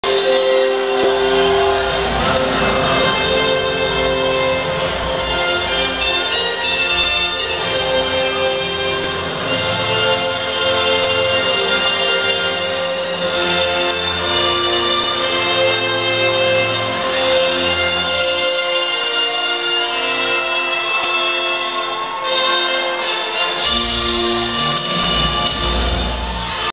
Lancer du mortier 2007 (Collège Durocher St-Lambert)
Lancer du mortier 2007 du Collège Durocher à la Place des Arts.